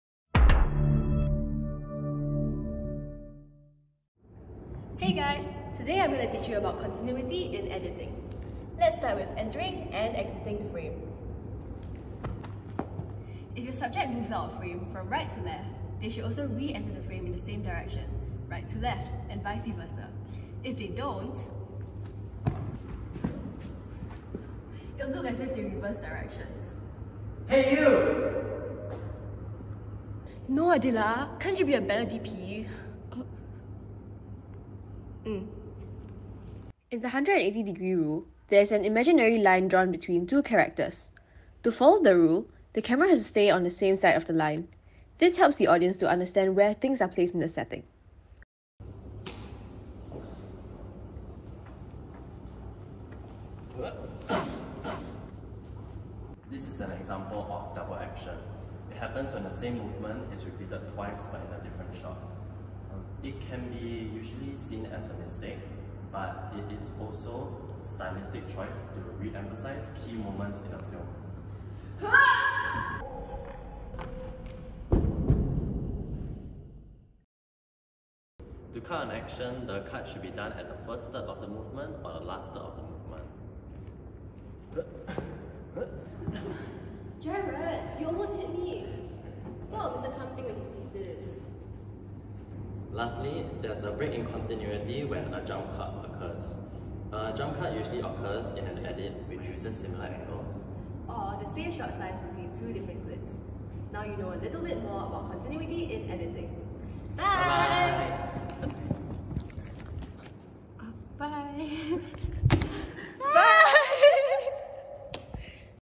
Continuity in Editing - Ngee Ann Polytechnic's Film, Sound & Video | Erica Lee YQ
7 Dec 2023

A group project for my Diploma's Editing module where we had to create a 1-2min 16:9 film to explain what continuity in editing is.

Director: Erica Lee YQ
Director of Photography: Nur Adilah
Fight Choreographer: Erica Lee YQ
Editors: Erica Lee YQ & Gerard Jie Meehan
Actors: Erica Lee YQ & Gerard Jie Meehan